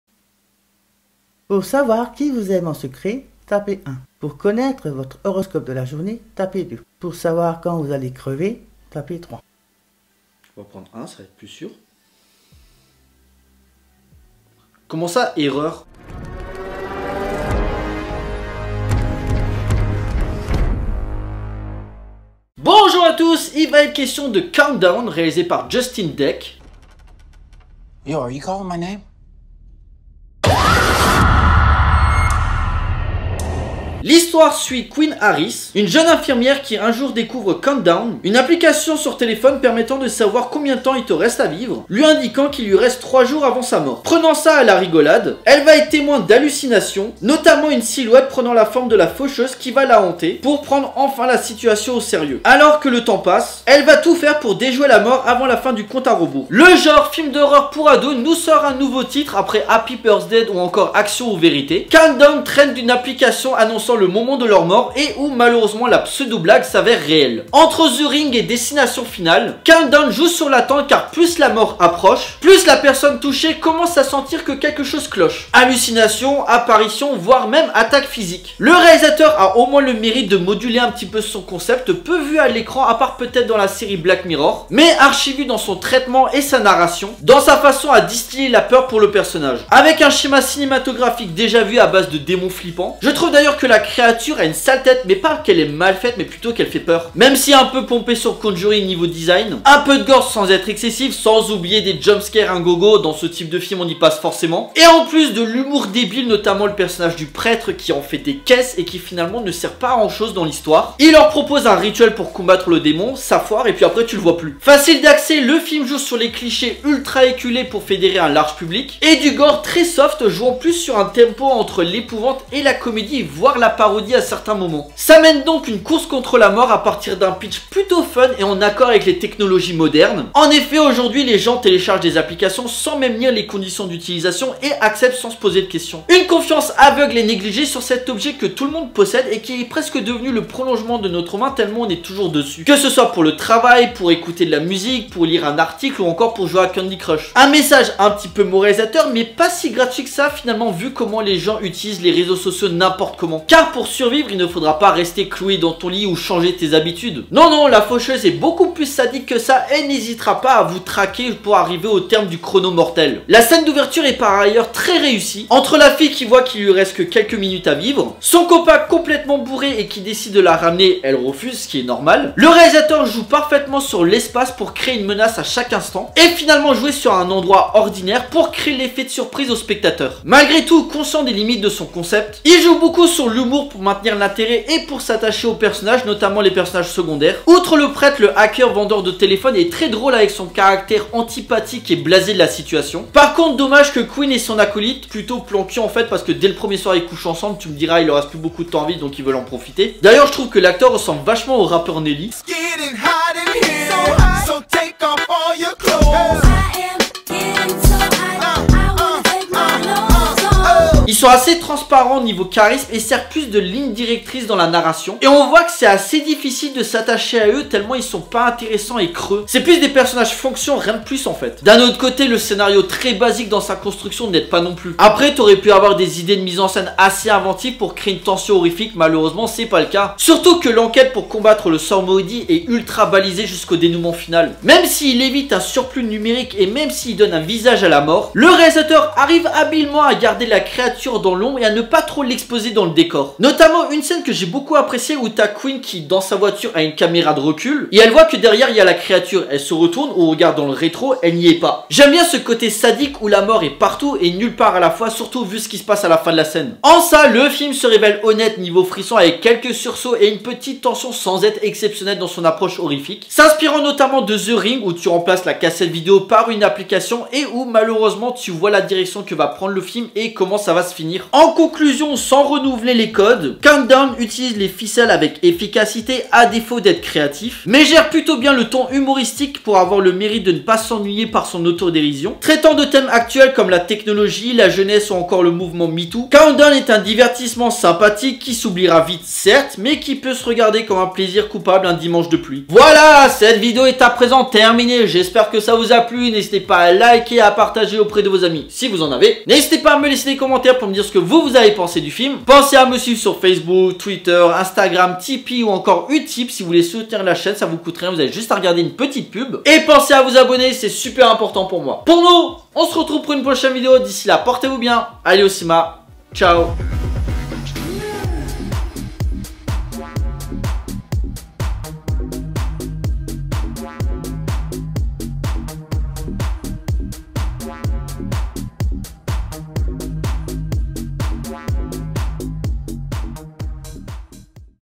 Vlog - Countdown
Yo les gens ça va ? Petit film d'horreur avec une application maléfique, dans le rayon horreur teen movies on a connu mieux mais malgré tout Countdown est-il mauvais pour autant ? Réponse dans la vidéo!!! Like et partage un max si tu as aimé la critique =)

Titre : Countdown
Réalisé par Justin Dec
Sociétés de production : Boies / Schiller Film Group Two Grown Men
Wrigley Pictures
Société de distribution : STX Entertainment
Pays d'origine : Etats Unis
Durée : 1h30min
Genre : Horreur
Date de sortie : 13 novembre 2019
Avec Elizabeth Lail, Peter Facinelli, Jordan Calloway & P.J. Byrne